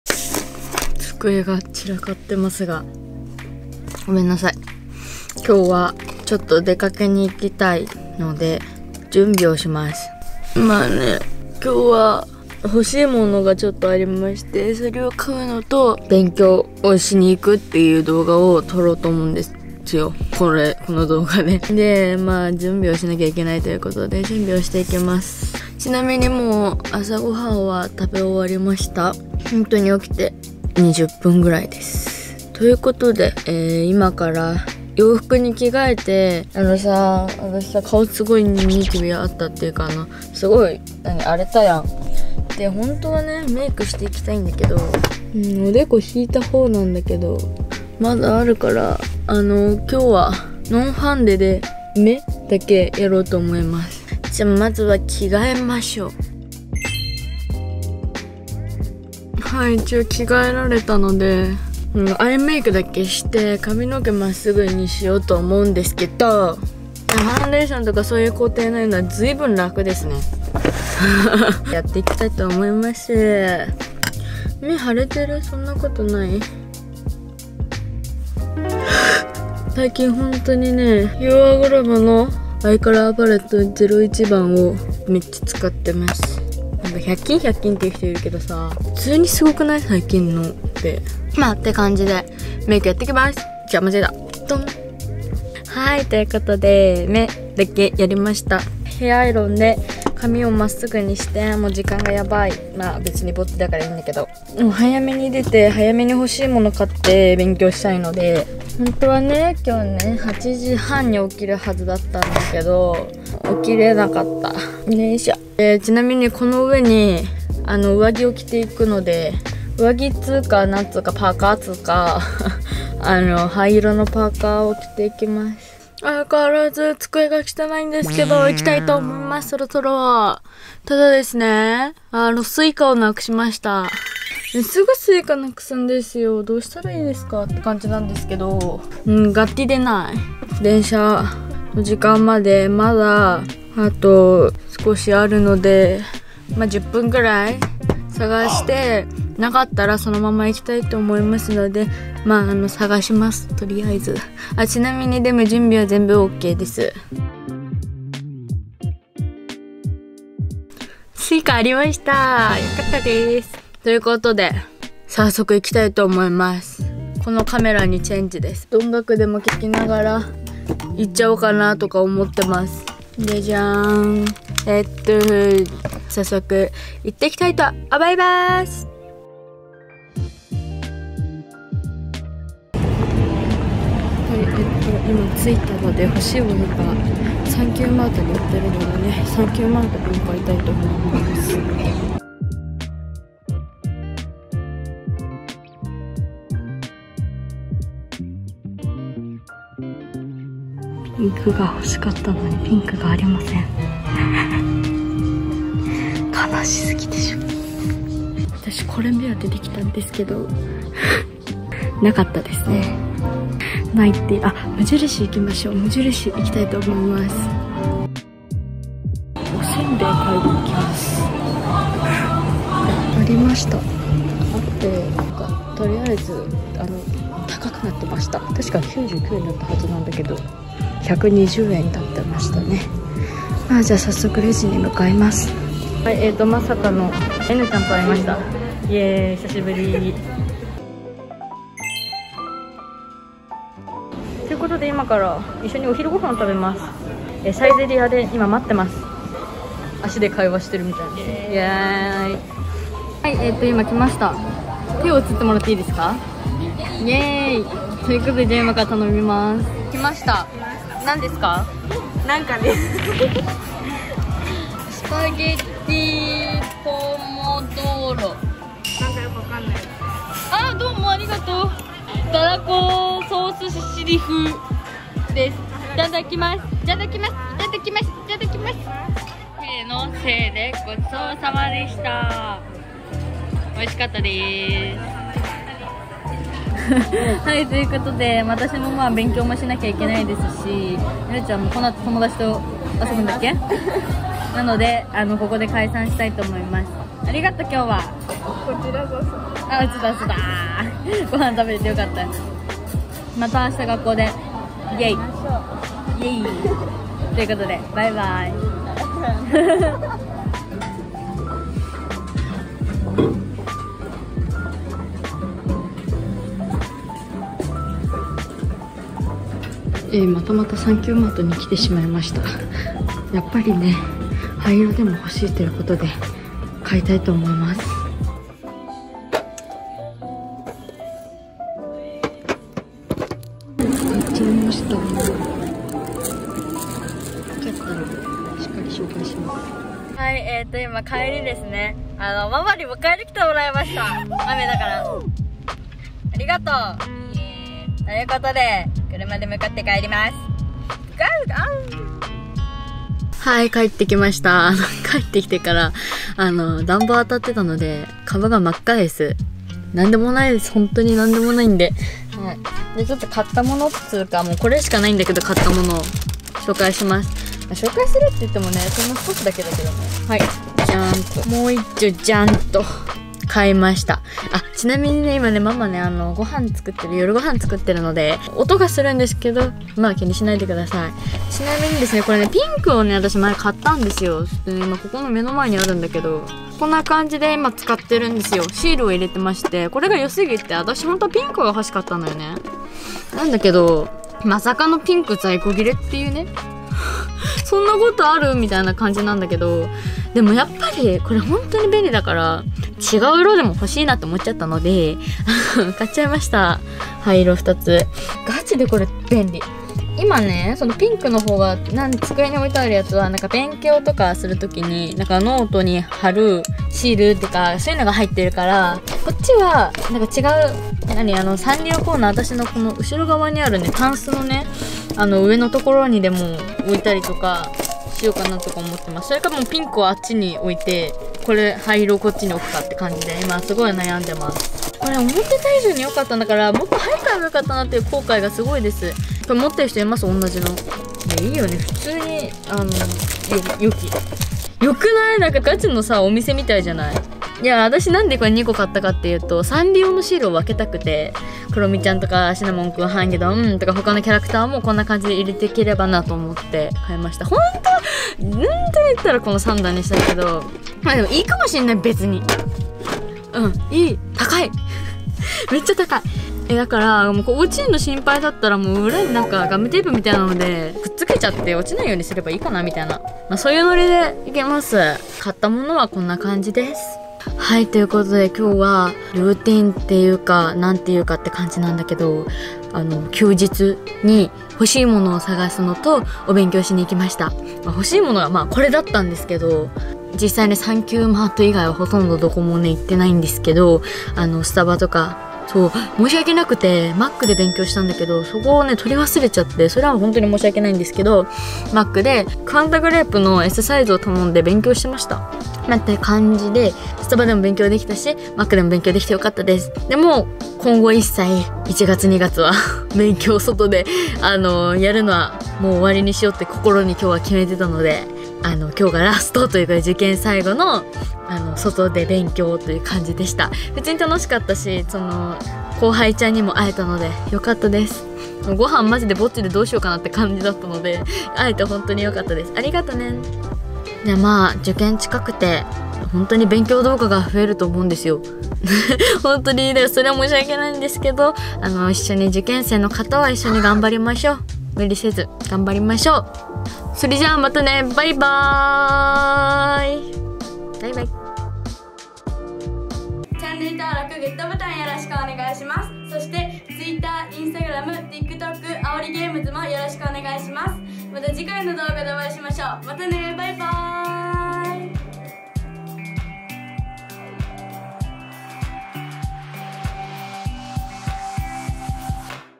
【受験生のルーティン】お外でお勉強＆お買物に行ってきます！お目当てのあの商品を買って、スタバ＆マックで勉強してきたよ！！
今回は受験生のルーティンをvlog的にご覧下さい♪
毎日部屋で勉強しているとモチベが下がってしまうので、大好きなスタバやマック図書館でお勉強しについでにちょっとお買物も。